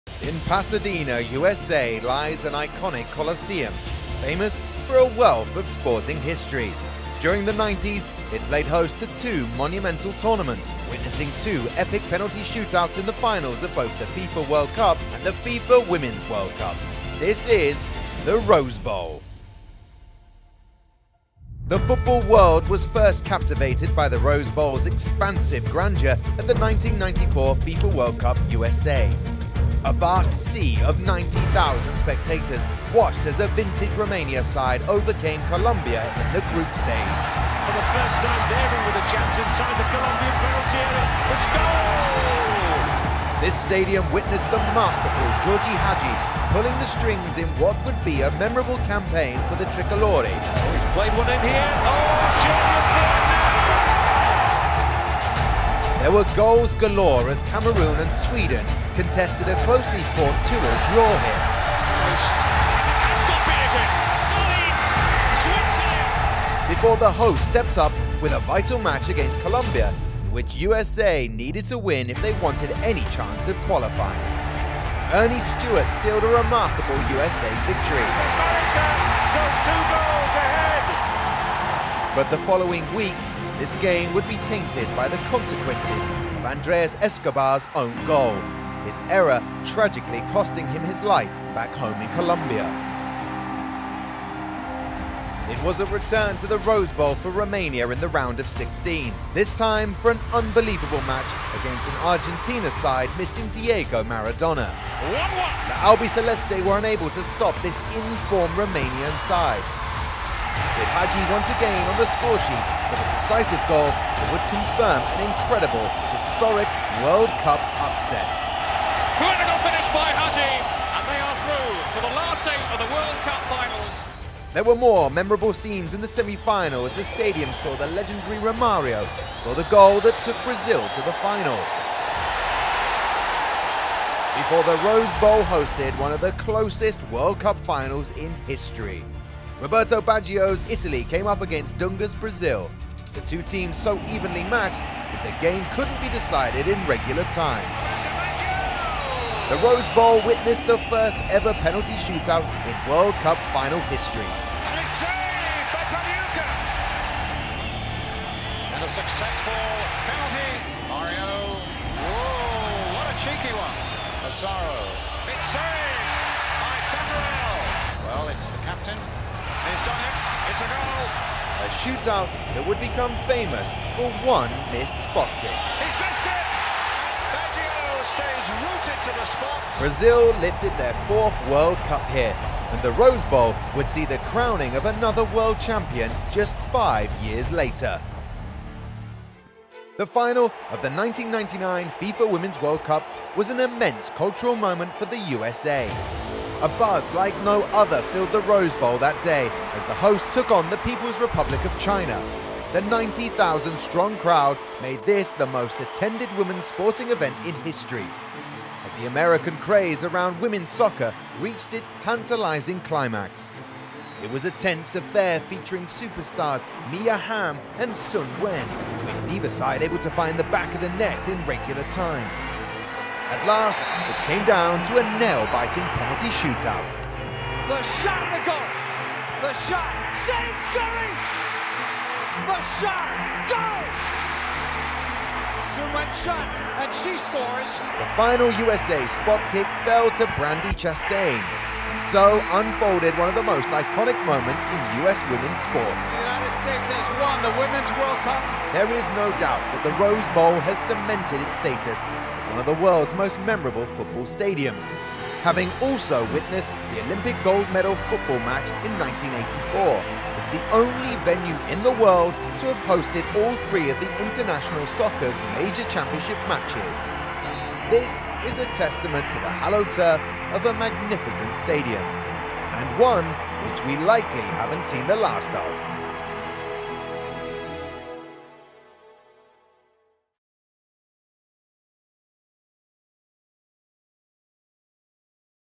Rose Bowl | USA 1994 & 1999 | FIFA World Cup & FIFA Women's World Cup
In Pasadena, USA lies an iconic colosseum famous for a wealth of sporting history. During the 90s, it played host to two monumental tournaments, witnessing two epic penalty shoot-outs in the finals of both the FIFA World Cup and the FIFA Women’s World Cup. This is: the Rose Bowl.

From Brazil lifting their fourth FIFA World Cup Trophy in 1994 to Brandi Chastain's iconic penalty and celebration for USWNT against China in 1999, the Rose Bowl has been the setting of many historic moments.